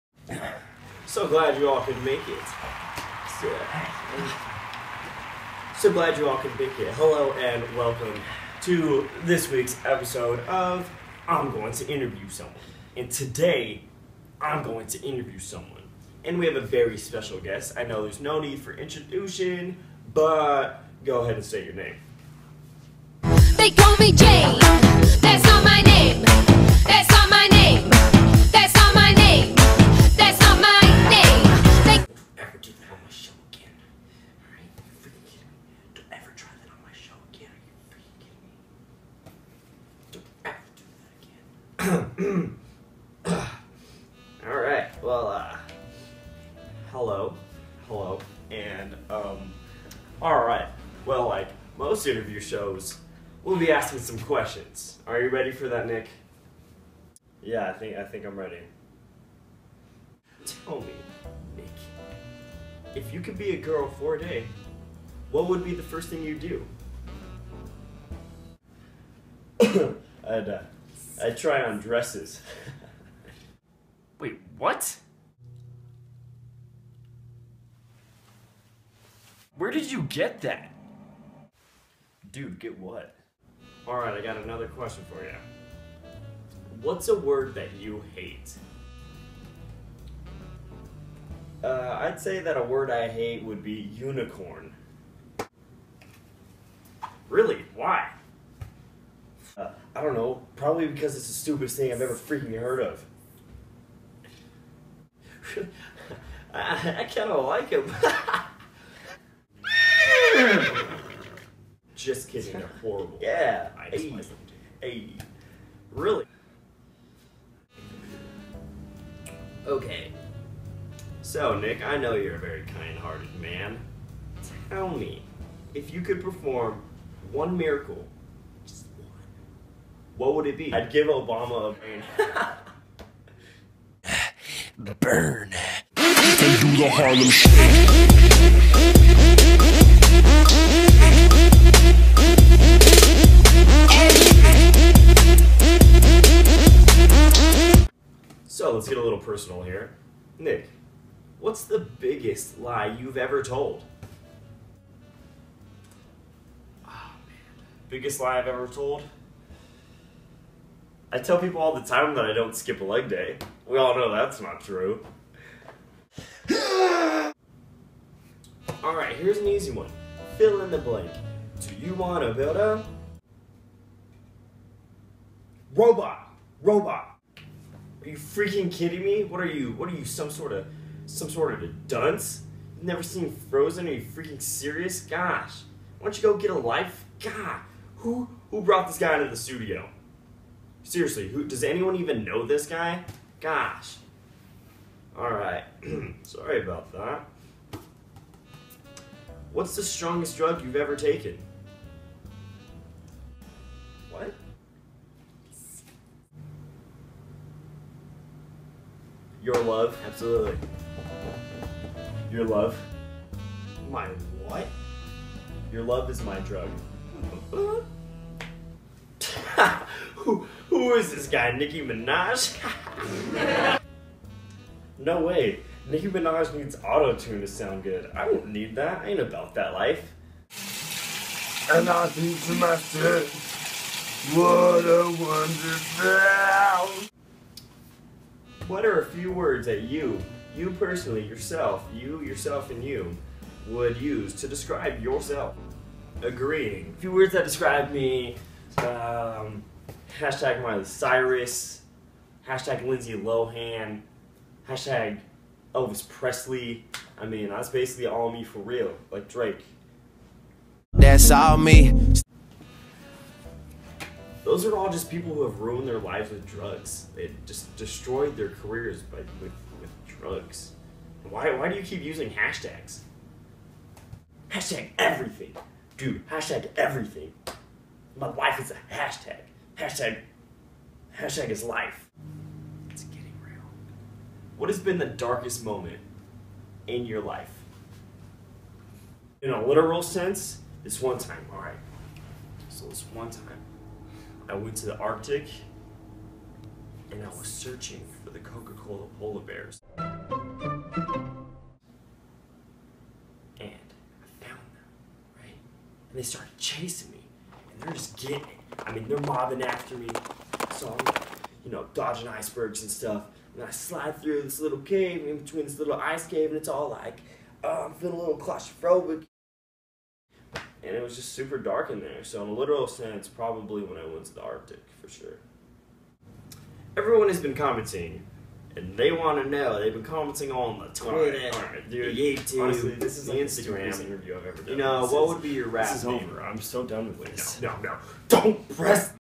I'm Going To Interview Someone
Just a little interview I conducted with myself. Hope you enjoyed. God Bless.